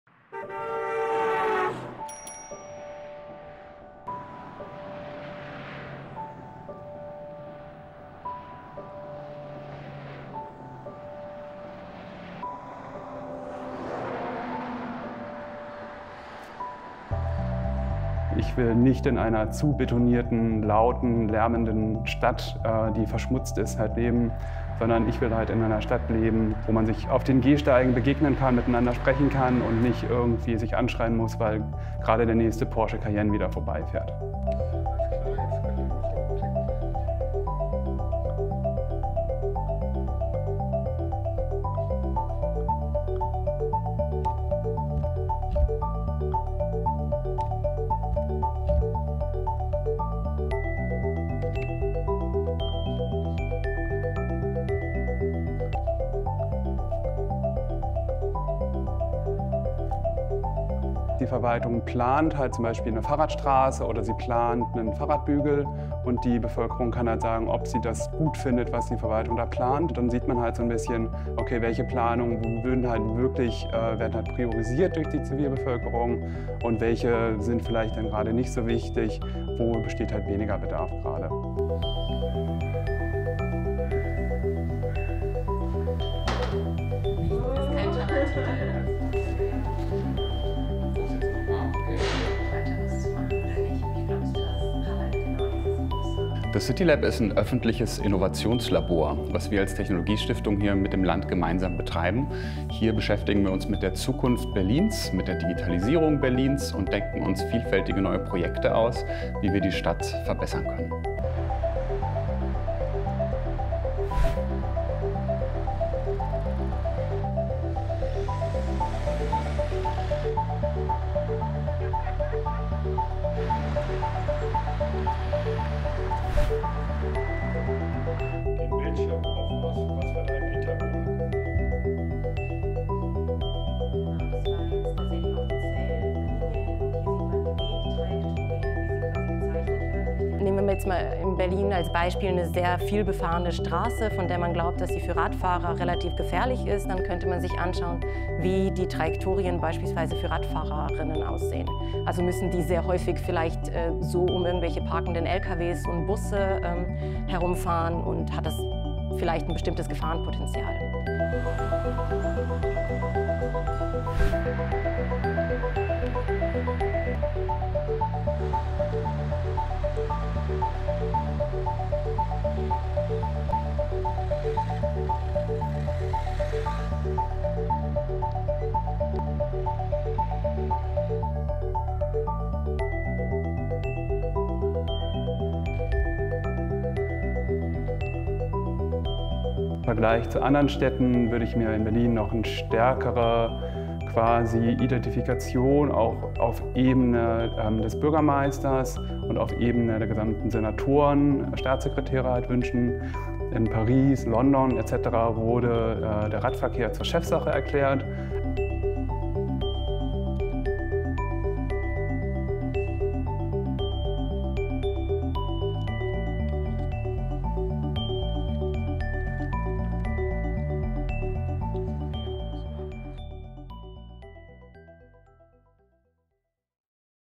Inovasi Ibu Kota Jerman untuk Atur Lalu Lintas di Berlin
Bagaimana cara agar warga ibu kota Jerman nyaman bertransportasi? CityLAB di Berlin menjadi laboratorium inovasi yang melahirkan ide-ide canggih agar bisa jadi kota berkelanjutan yang lebih hijau dan cikal bakal kota masa depan yang nyaman. Inovasi CityLAB dalam mengatur lalu lintas seperti misalnya elwat Fix My Berlin jadi mendesak karena populasi Berlin meningkat sekitar 20.000 setahun. Beberapa orang memperkirakan, kota ini akan dihuni 4 juta orang pada tahun 2030.

Suka dengan video DWGlobalIdeas ini?